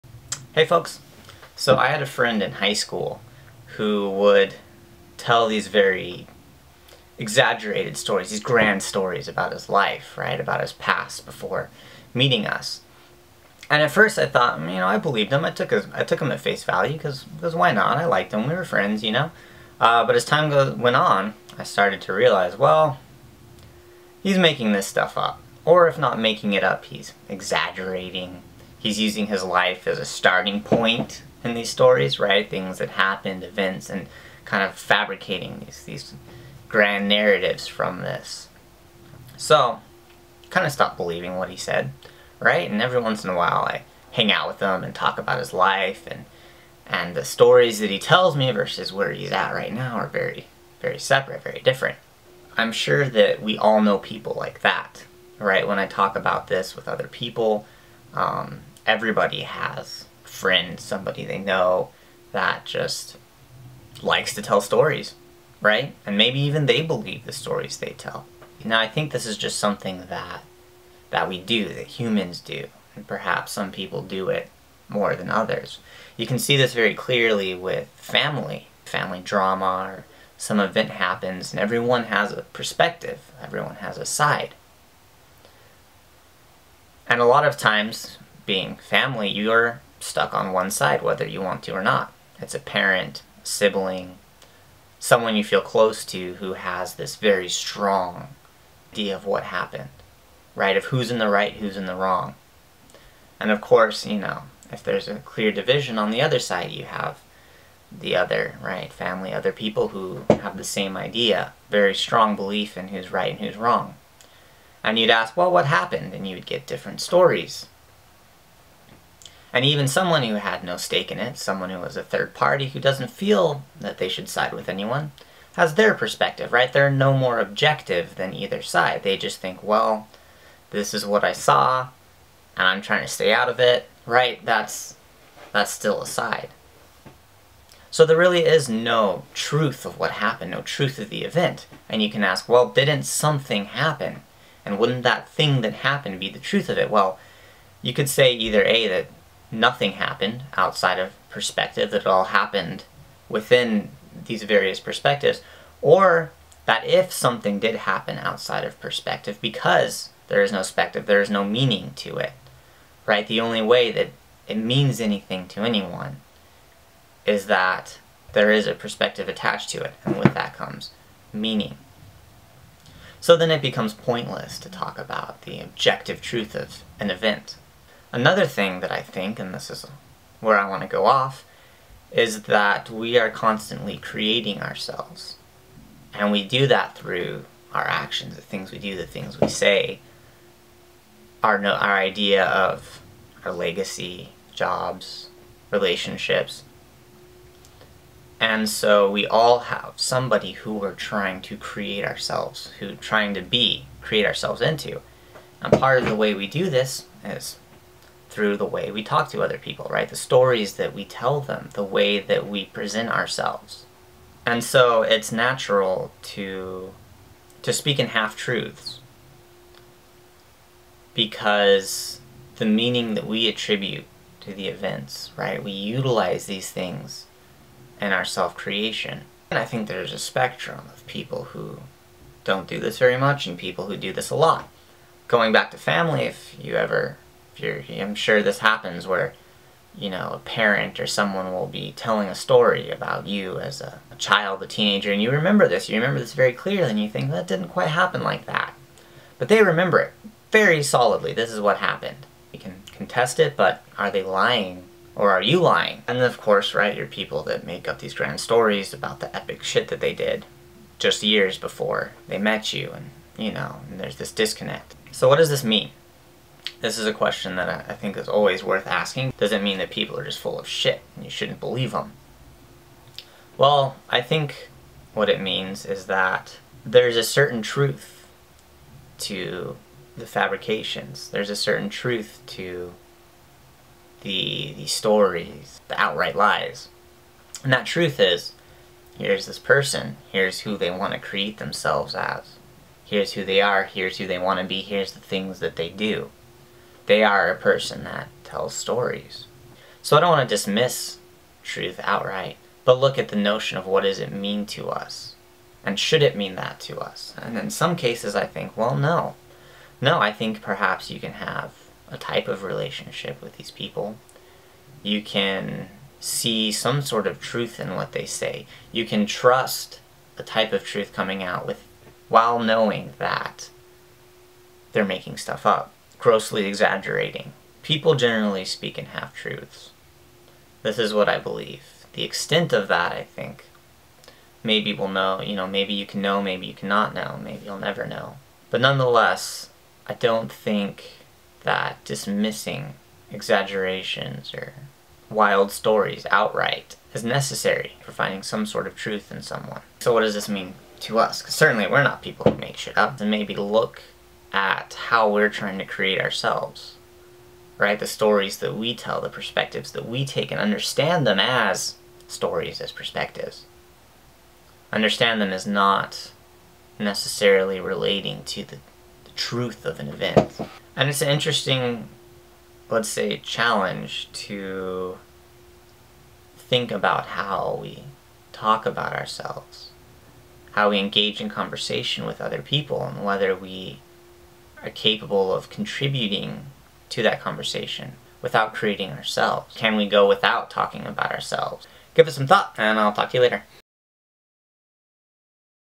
Why people make shit up
A look into the exaggerated stories that we hear from others and quite possible the ones we make ourselves. Why this happens and what we should do about it.